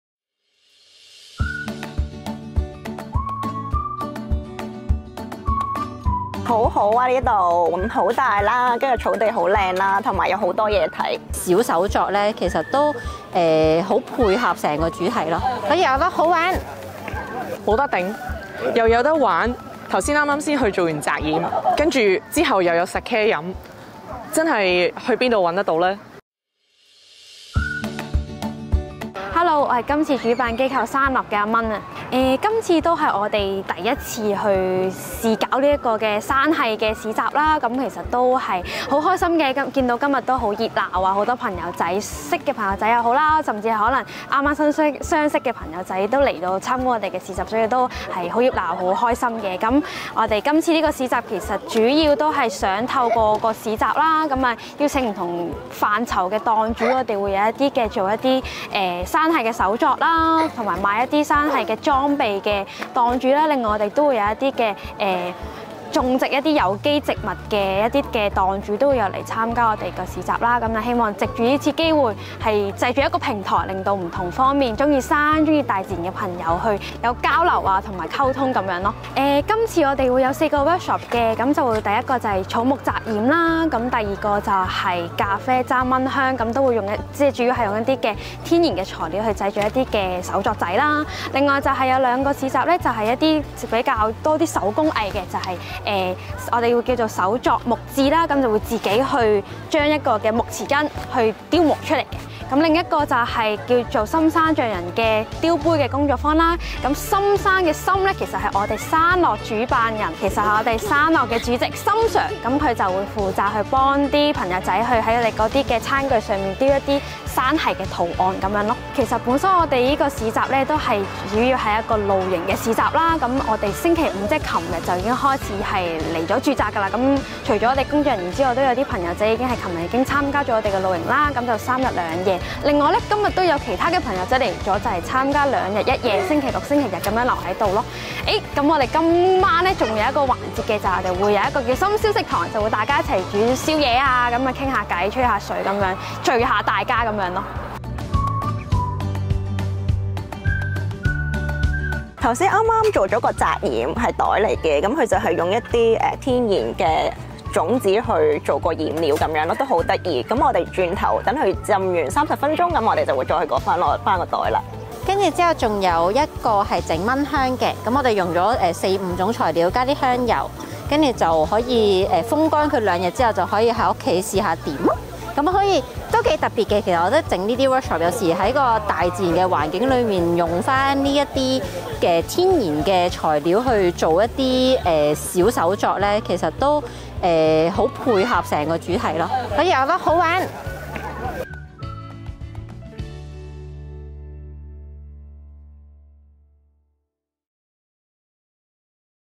山中有樂 露營市集 工作坊 錦田Ease Camp 宣傳露營文化
露營市集 主辦 : 山中有樂 工作坊 宣傳露營文化
地點 : 錦田 Ease Camp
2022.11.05

GoOut Couple 周老闆和周夫人的分享 戶外裝備 | 場地 | 飲食 | 活動
享受大自然、愛護大自然、自己垃圾自己掉